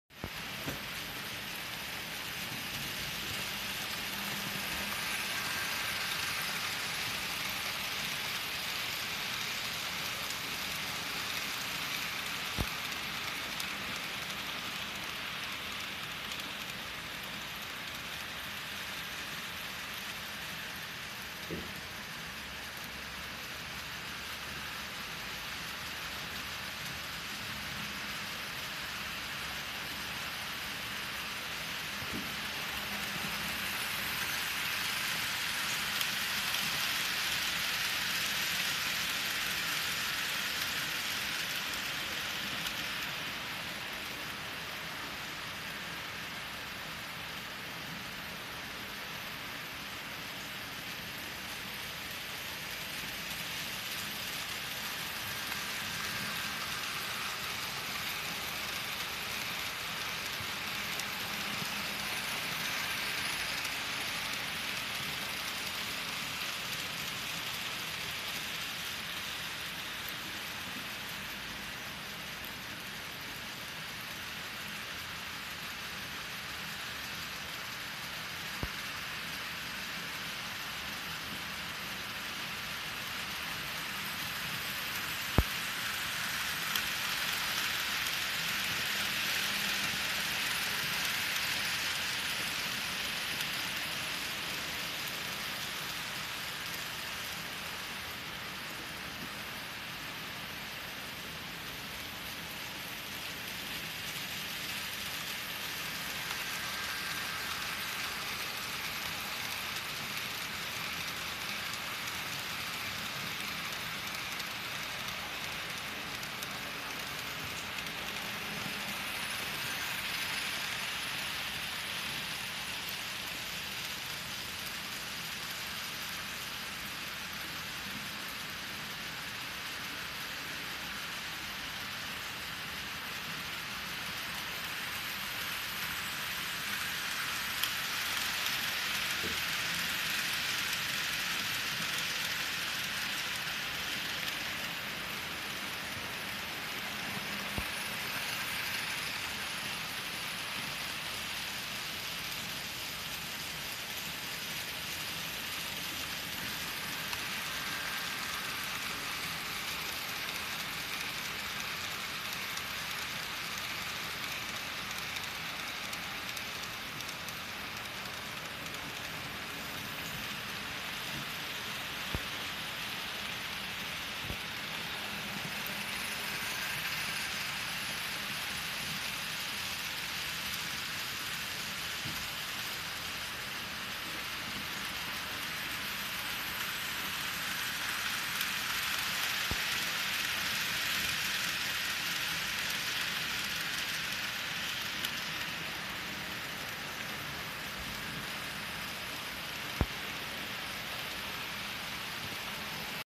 v0016 - Trains passing by #15
View down the line - 3 trains including my Amtrak passenger for the first time.